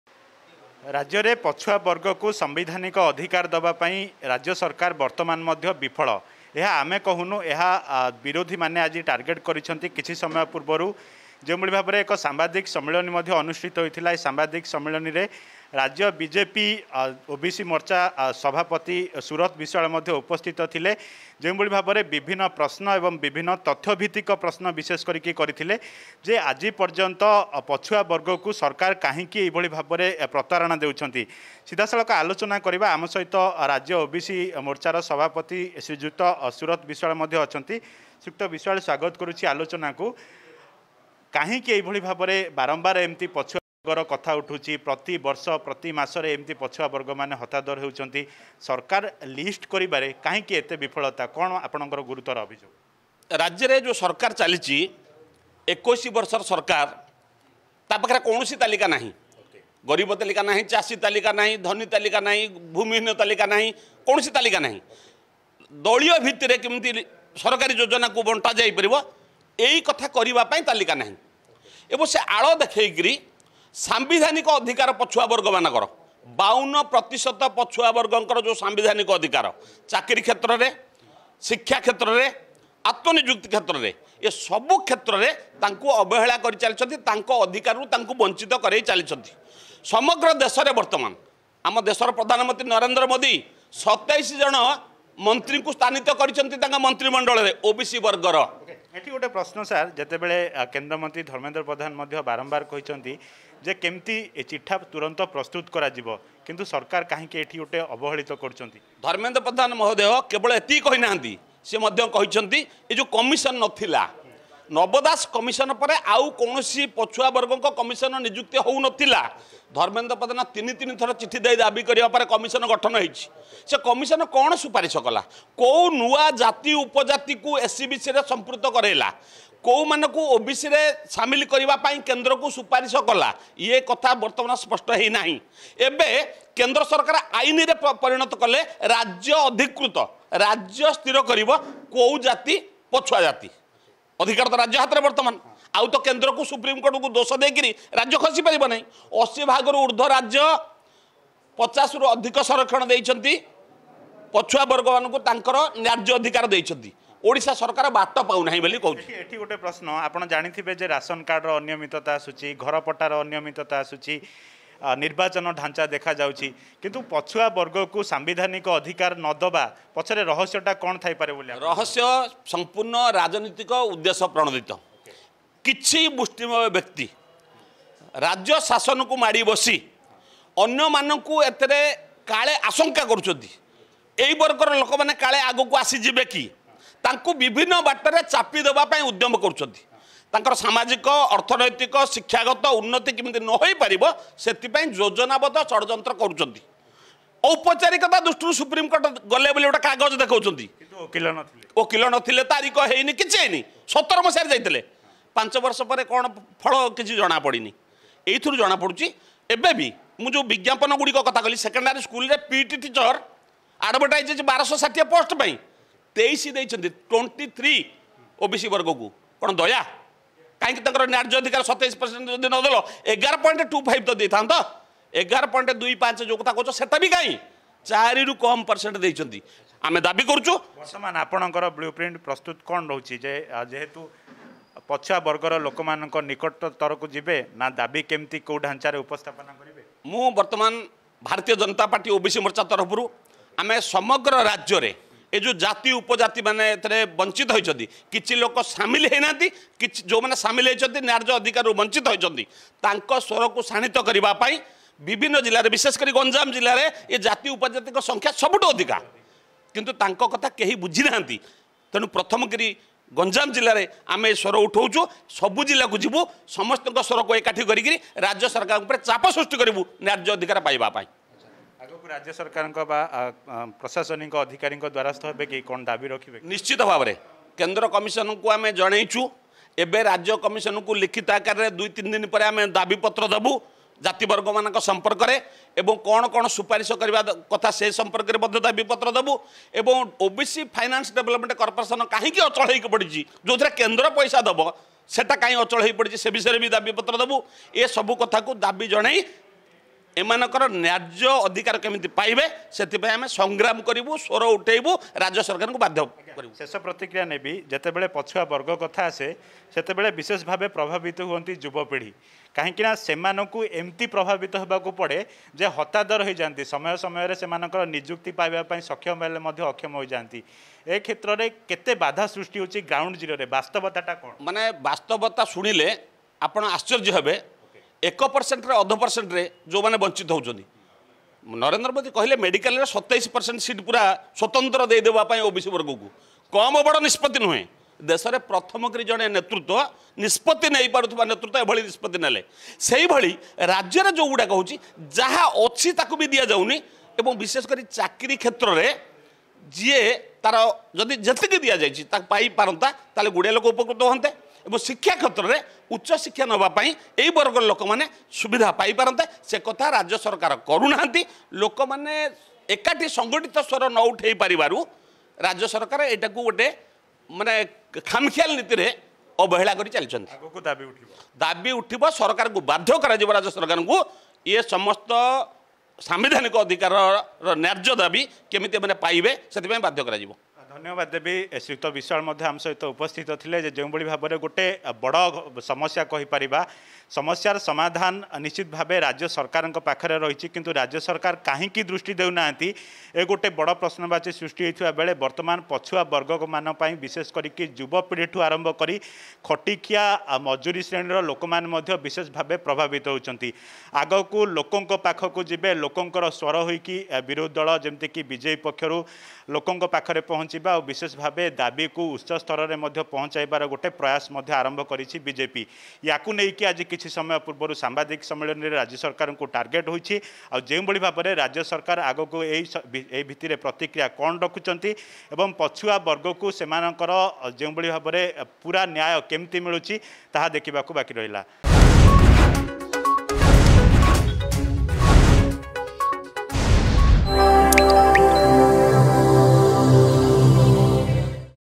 ରାଜ୍ୟରେ ପଛୁଆବର୍ଗ କୁ ନେଇ ପୁଣି ତେଜିଲା ଅସନ୍ତୋଷ।
ରାଜ୍ୟରେ ପଛୁଆବର୍ଗ କୁ ନେଇ ପୁଣି ତେଜିଲା ଅସନ୍ତୋଷ। ଏନେଇ ରାଜ୍ୟ ବିଜେପି କରିଛି ଗୁରୁତର ଅଭିଯୋଗ ।